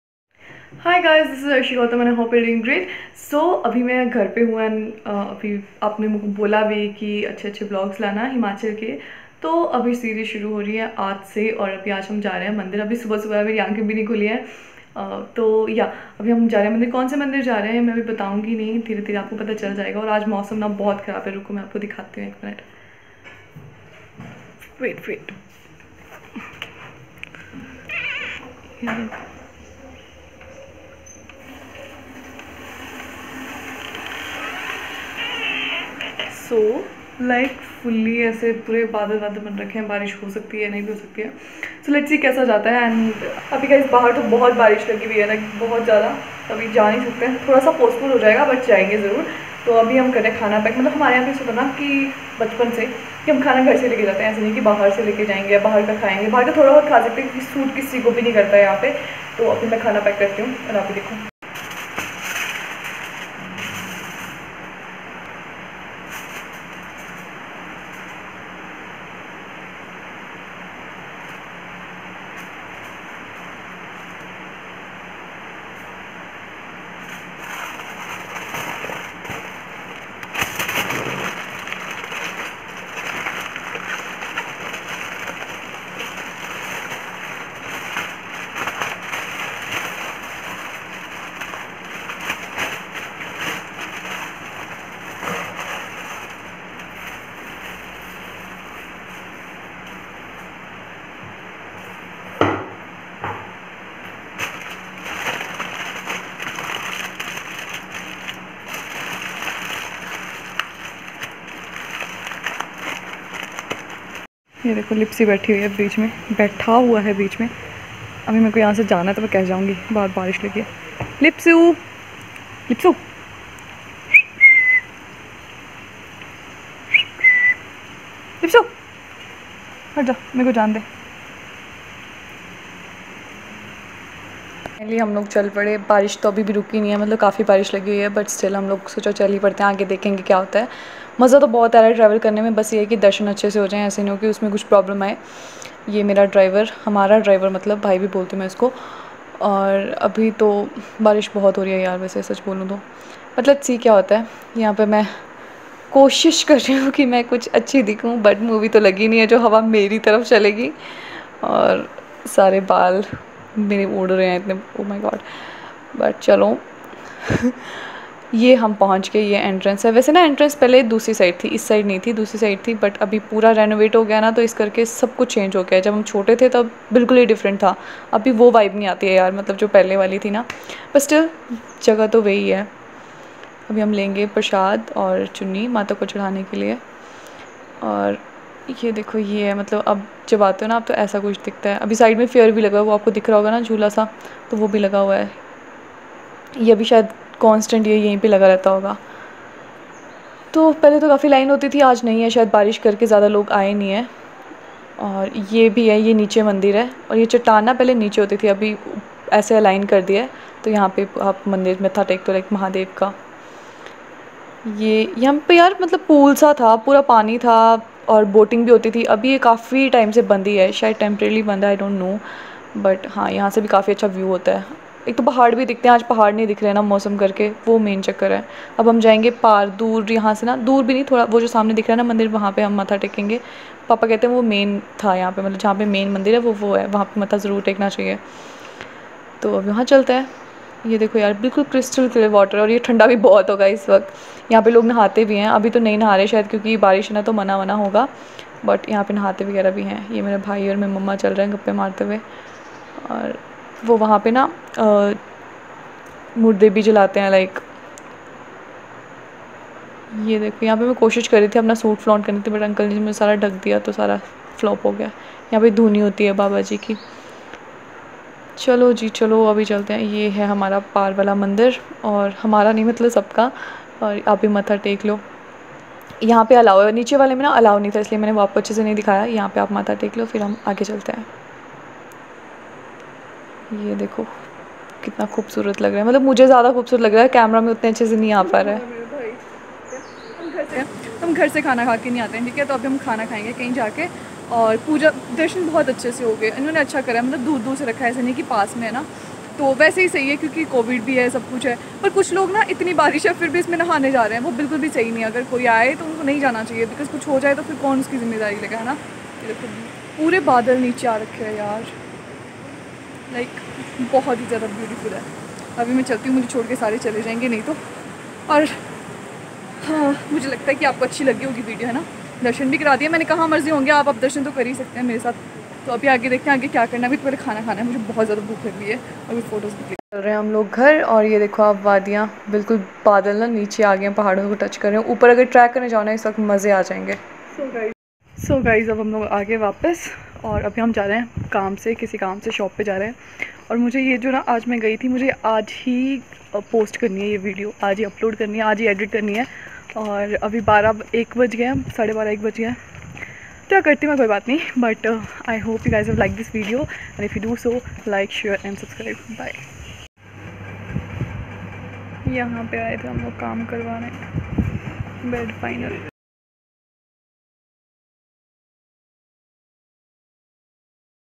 A Trip with Family || Chamunda Devi|| Vlog || Most Beautiful place 🌻|| Himachal Series || Vlog 1
I went to Maa chamunda devi temple which is himachal and is one among the most holy and beautiful places i hope you. liked my attempt of showing ypu this amazing shrine .